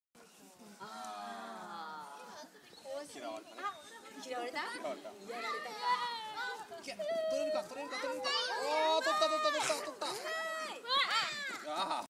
大きなシャボン玉
子どもたちは大喜びでした。